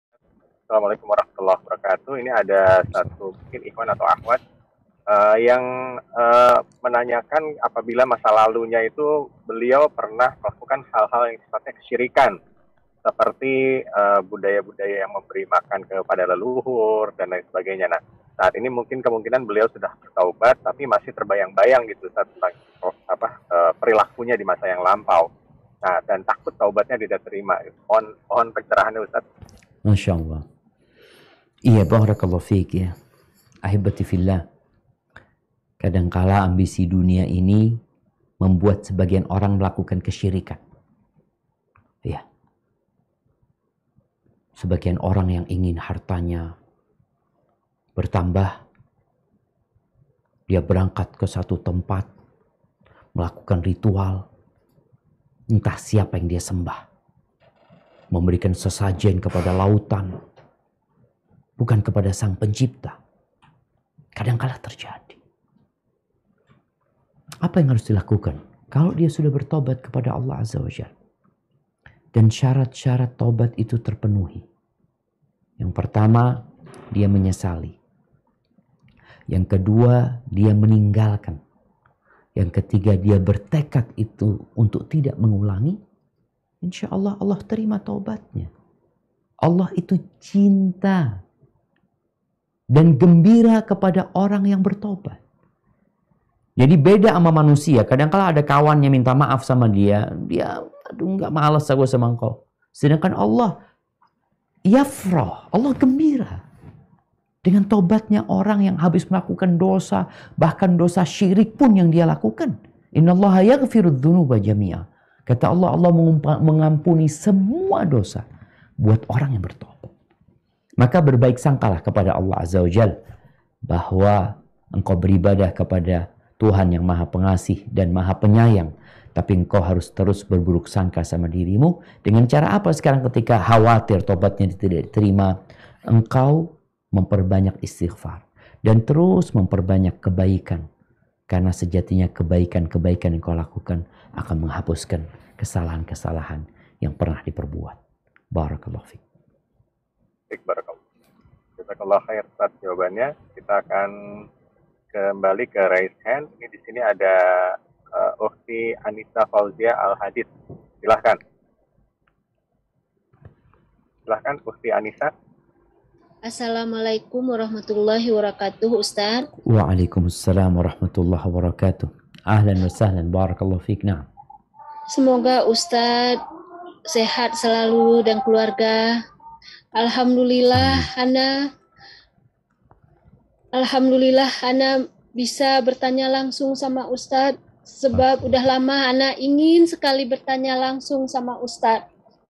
BAGAIMANA DENGAN ORANG YANG DULU PERNAH MELAKUKAN KESYIRIKAN SEPERTI MEMBERI MAKAN ARWAH LELUHUR
Assalamu'alaykum, Silahkan Share jika ada manfaatnya.
Judul Kalian Full : Lebih Merusak Daripada Serigala Lapar - Ustadz Dr Syafiq Riza Basalamah, M.A
Sumber Youtube : Syafiq Riza Basalamah Official